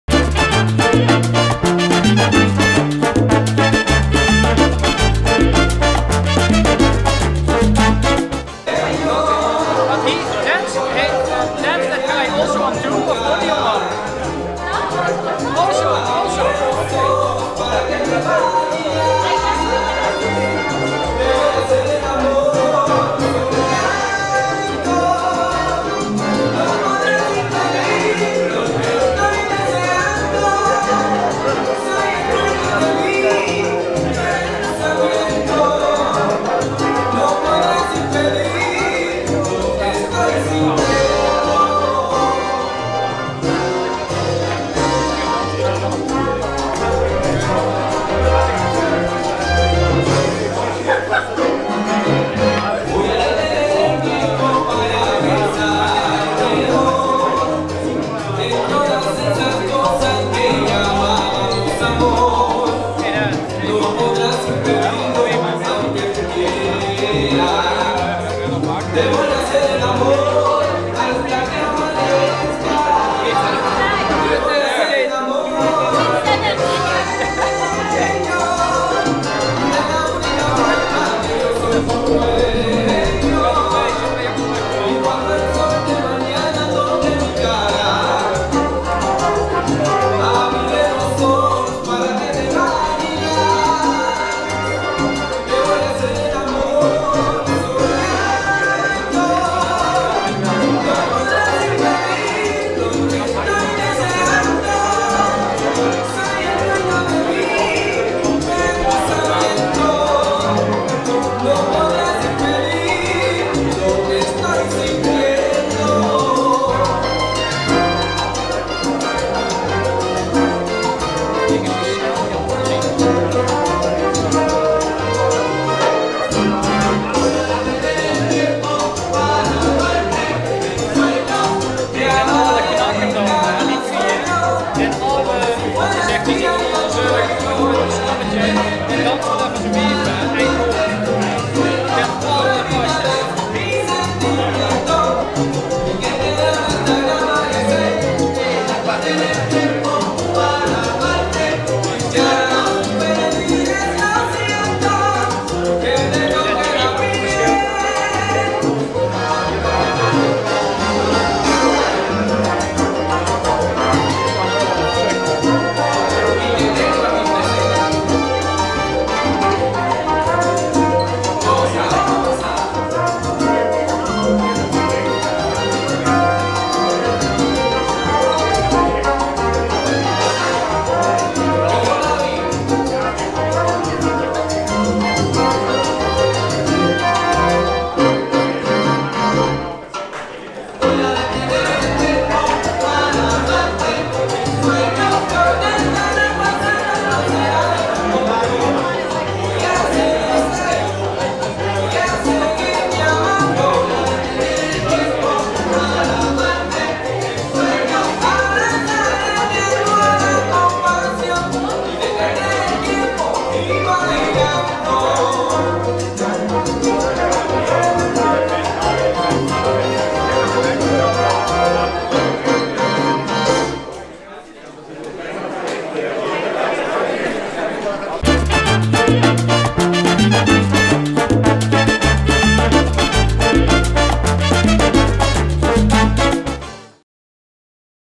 Jonathan & Cecile Dancing Salsa @ El Sol Warsaw Salsa Festival 2019
Dancers: Jonathan & Cecile
Congress: El Sol Warsaw Salsa Festival 2019
Videographer: Salsalandis